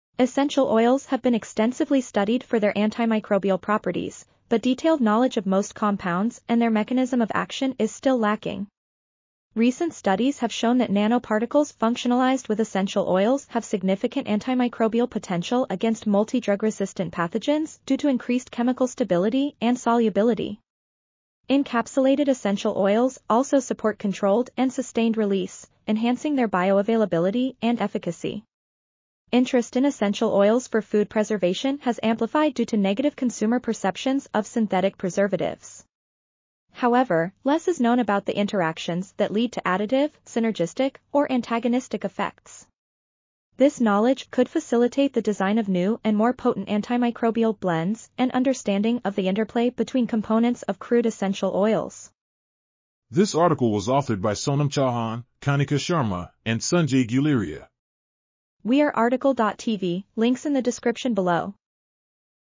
Antimicrobial Activity of Some Essential Oils—Present Status and Future Perspectives | RTCL.TV
### Keywords ###

### Article Attribution ###
Title: Antimicrobial Activity of Some Essential Oils—Present Status and Future Perspectives
Authors: Sonam Chouhan, Kanika Sharma ,and Sanjay Guleria
Publisher: MDPI AG
DOI: 10.3390/medicines4030058

### Video Timestamps ###
0:00:00 - Summary
0:00:52 - Title
0:00:58 - Tail
0:01:02 - End